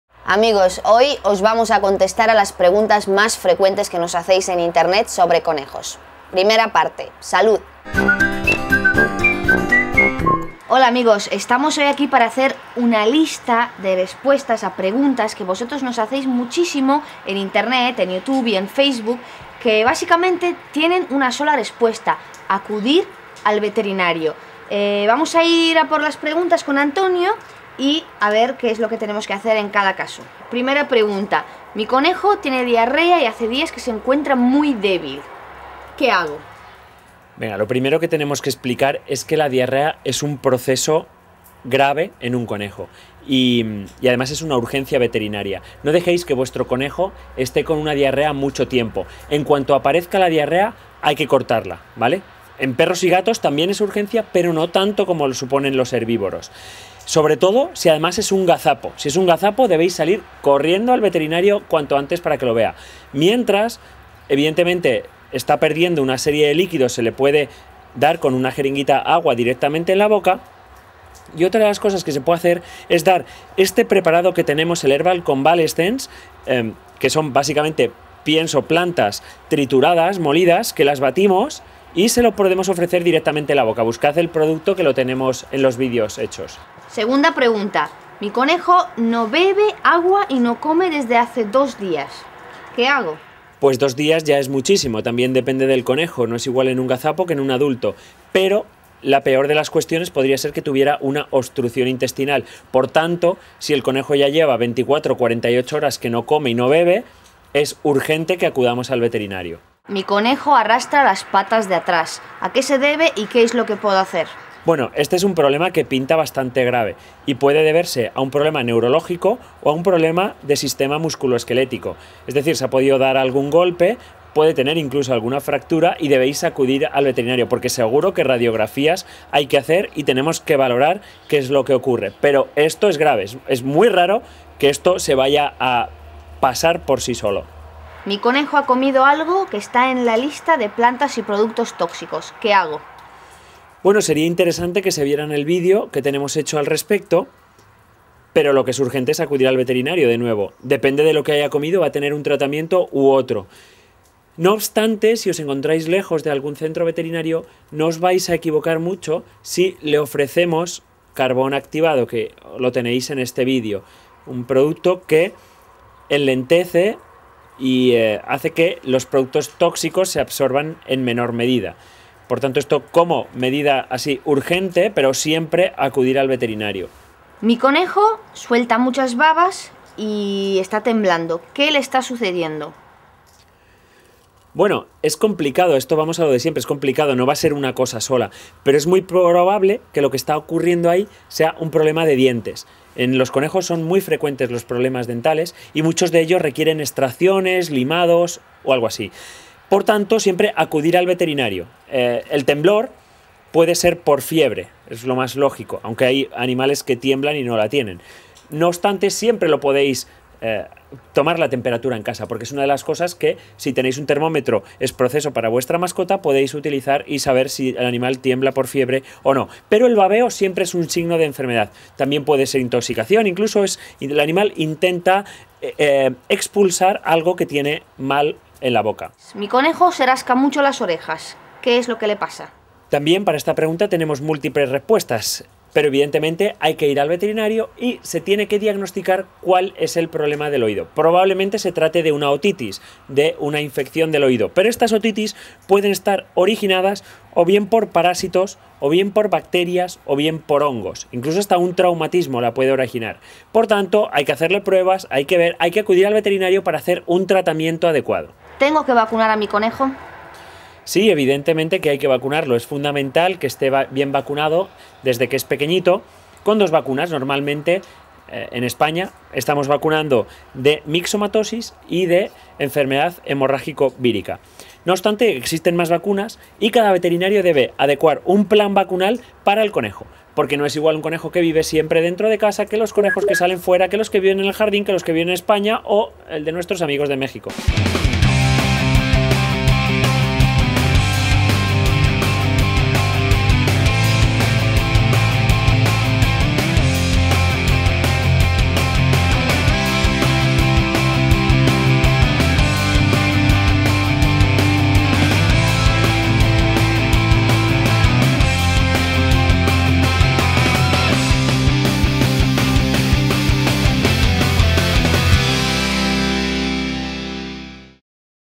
CONEJOS - Preguntas frecuentes sobre la salud de los conejos. Respuestas del veterinario.
En este vídeo de EL CÓMO Y EL PORQUÉ DE MI MASCOTA os
Muchas de las cuestiones son comunes, como los casos de diarrea, los casos de falta de apetito, obstrucción intestinal, problemas con las patas de atrás, intoxicaciones, salivación excesiva, picores, vacunas, problemas de dientes... Siempre debéis acudir al veterinario, pero no está de más realizar alguna medida de urgencia en casa como las que nos cuenta nuestro veterinario Antonio Rodríguez. ¡Atentos! Si quereis encontrar nuestros productos no dudeis en visitar nuestra web